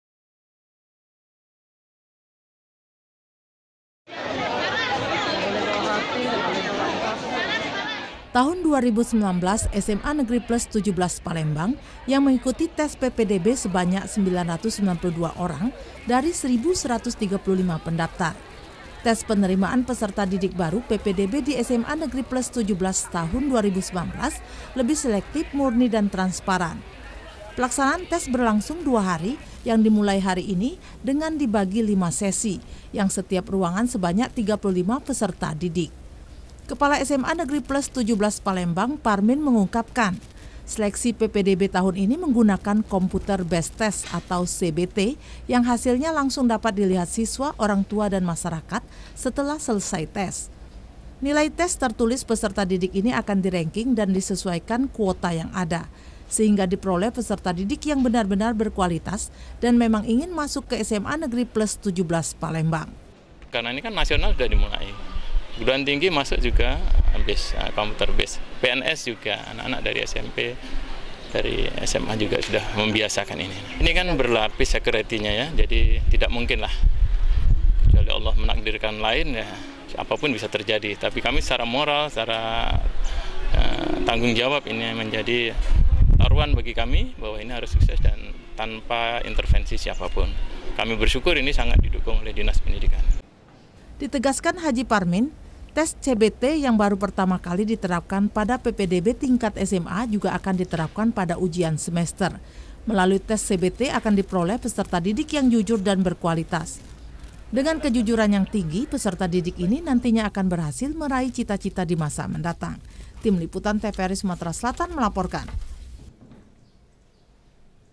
SELEKSI KETAT PPDB SMA PLUS NEGERI 17 PALEMBANG
Warta Sumsel TVRI, 21 Januari 2019
Rep/Cam : Aan Muzhar/Rita